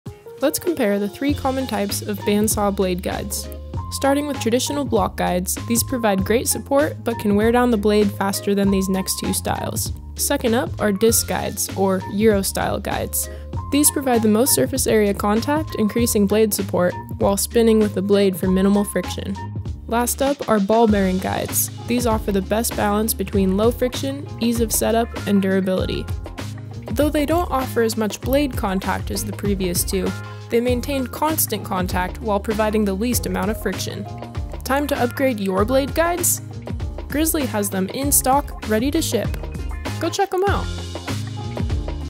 Do you know the 3 common types of bandsaw blade guides?
Bandsaw Blade Guides come in different shapes and sizes. Here are the 3 common types. What is your go-to?